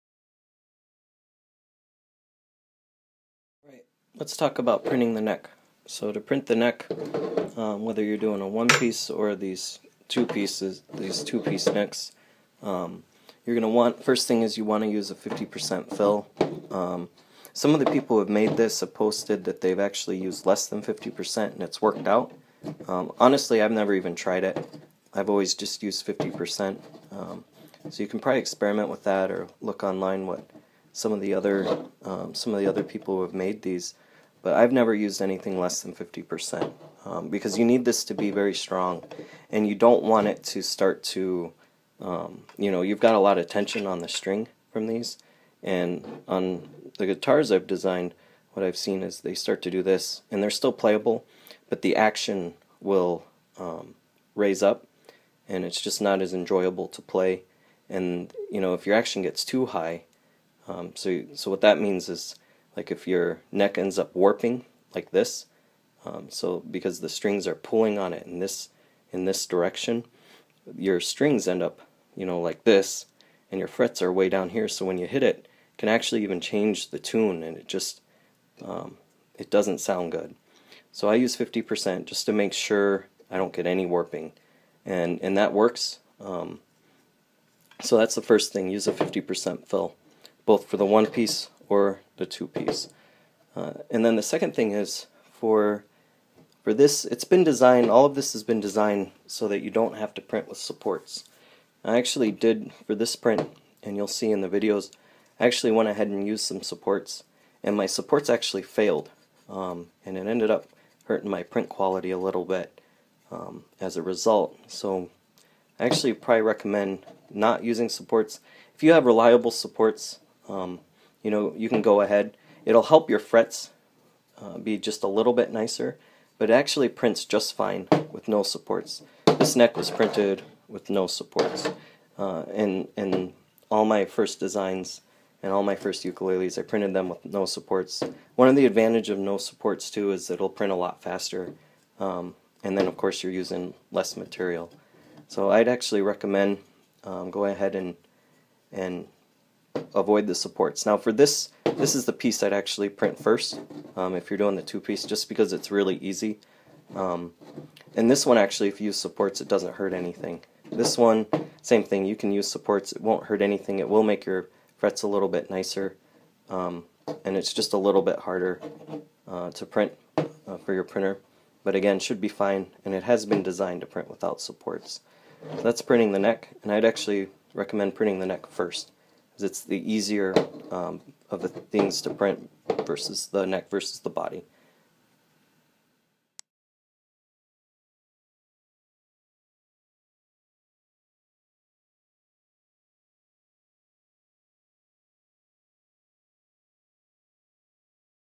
Make a 3D printed Ukulele - Part 3
This is part 3 in a series explaining how to 3D print your own Ukulele - Printing the Neck.

You can find the directions and files on